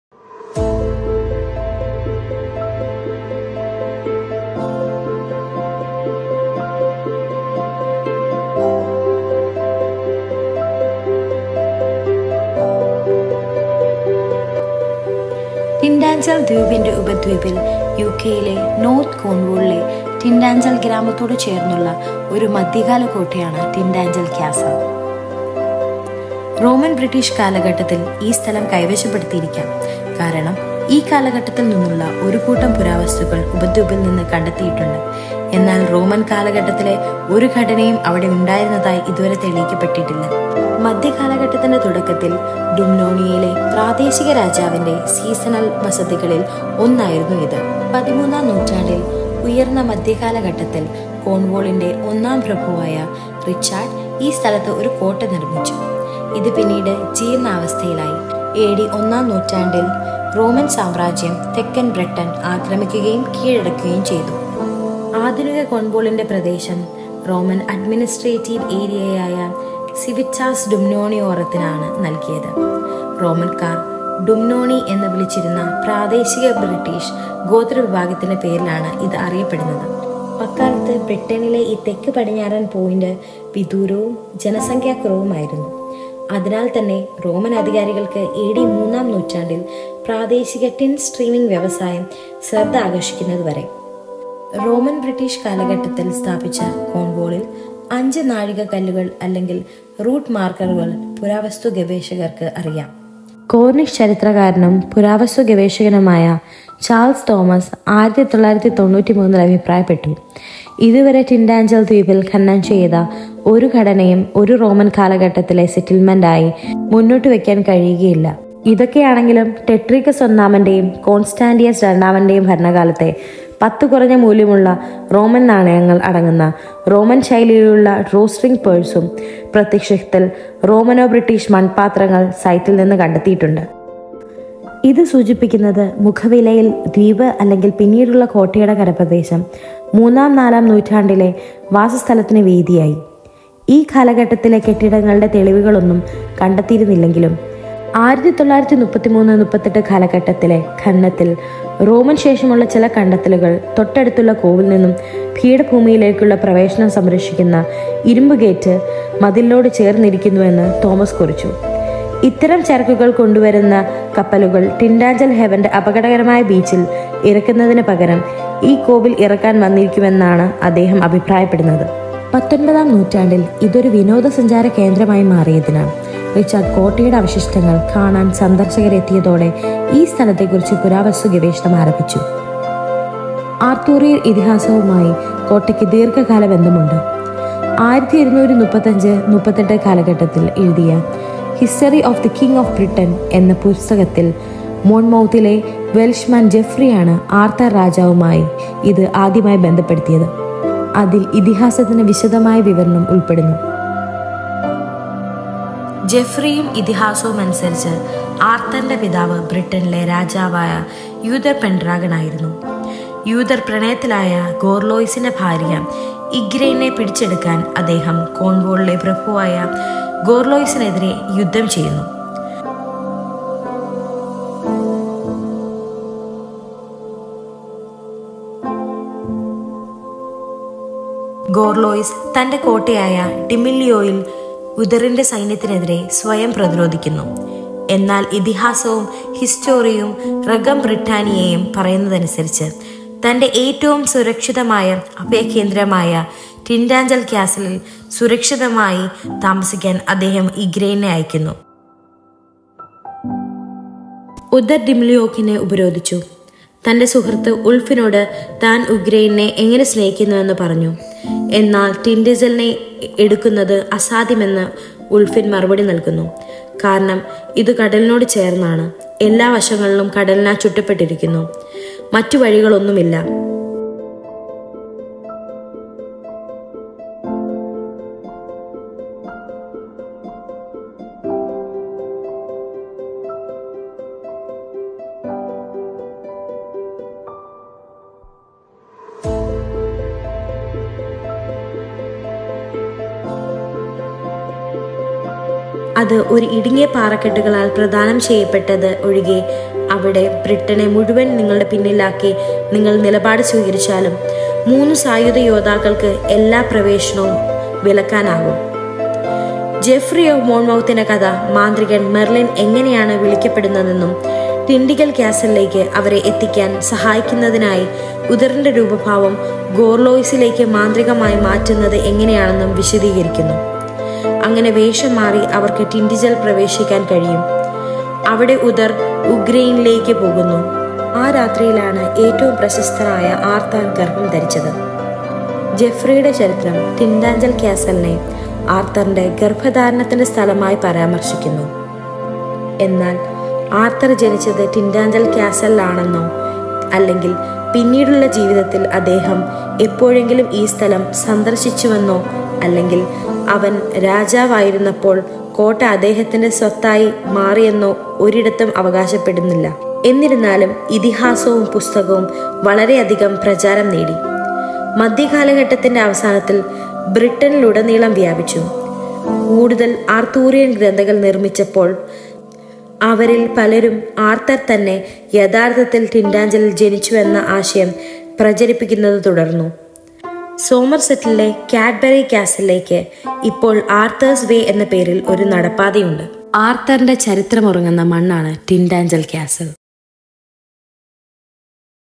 Tintagel Castle I Cornwall | UK | Malayalam
Tintagel Castle  is a medieval fortification located on the peninsula of Tintagel Island adjacent to the village of Tintagel (Trevena), North Cornwall in the UK. The site was possibly occupied in the Roman-British period, as an array of artefacts dating from this period have been found on the peninsula, but as yet no Roman-era structure has been proven to have existed there. It was settled during the early medieval period, when it was probably one of the seasonal residences of the regional king of Dumnonia. A castle was built on the site by Richard, 1st Earl of Cornwall in the 13th century, during the High Middle Ages. It later fell into disrepair and ruin.

In the 1st century AD, southern Britain was invaded and occupied by the Roman Empire. The territory of modern Cornwall was assigned to the Roman administrative region of civitas Dumnoniorum, named after the local British tribal group whom the Romans called the Dumnonii. At the time, this south-westerly point of Britain was "remote, under-populated ... and therefore also unimportant [to the Roman authorities] until, during the 3rd century AD, the local tin-streaming industry attracted attention." Archaeologists know of five milestones or route-markers in Cornwall erected in the Romano-British period. Two of these are in the vicinity of Tintagel, indicating that a road passed through the locality.

Cornish historian and archaeologist Charles Thomas noted in 1993: "So far, no structure excavated on Tintagel Island... can be put forward as a Roman-period settlement, native-peasant or otherwise." Despite this, a quantity of apparently Romano-British pottery has been unearthed on the site, as has a Roman-style drawstring leather purse containing ten low denomination Roman coins dating between the reigns of Tetricus I  and Constantius II. This suggests that "at face-value ... either the Island or the landward area of the later Castle formed the scene of third-fourth century habitation" even if no evidence has been found of any buildings dating from this period. Thomas also noted that some of the post-Roman finds in the excavations of 1933–38 were close to the wall known as the Iron Gate which guards access to the plateau from the adjacent cove. He suggests that the vessels bringing such goods might have come to unload at this cove rather than on the dangerous beach of Tintagel Haven.

Archaeological investigation into the site began in the 19th century as it became a tourist attraction, with visitors coming to see the ruins of Richard's castle.

The castle has a long association with the Arthurian legends; it was first associated with King Arthur by Welshman Geoffrey of Monmouth in his book Historia Regum Britanniae ("History of the Kings of Britain'"), written around 1135–38, which includes a detailed account of the legend. According to Geoffrey and the legend, Arthur's father was Uther Pendragon, the king of all Britain. He goes to war against Gorlois, the Duke of Cornwall, to capture Gorlois' wife Igraine, with whom Uther has fallen in love. Gorlois defends himself against Uther's armies at his fort of Dimilioc, but he sends Igraine to stay safely within Tintagel Castle which is his most secure refuge, according to the legend and Historia Regum Britanniae. Uther besieges Dimilioc, telling his friend Ulfin how he loves Igraine, but Ulfin replies that it would be impossible to take Tintagel, for "it is right by the sea, and surrounded by the sea on all sides; and there is no other way into it, except that provided by a narrow rocky passage—and there, three armed warriors could forbid all entry, even if you took up your stand with the whole of Britain behind you." Geoffrey of Monmouth's story goes on to explain how the wizard Merlin is summoned and magically changes Uther's appearance to that of Gorlois to help get them into Tintagel Castle, while also changing his own and Ulfin's appearances to those of two of Gorlois' companions. Disguised thus, they are able to enter Tintagel where Uther goes to Igraine, and "in that night was the most famous of men, Arthur, conceived

Geoffrey's History mentions Tintagel Castle as the site of Arthur's conception, but "it nowhere claims that Arthur was born at Tintagel, or that he ever visited the place in later life, or that in any sense the stronghold became his property when he was king." However, the legend and the book continued to become hugely popular, spreading across Britain in the Late Medieval period, when more Arthurian texts were produced, many of them continuing to propagate the idea that Arthur himself was actually born at Tintagel. There is now a footpath from the site to Cadbury Castle in Somerset called Arthur's Way.

Regards,
Kaylin’s World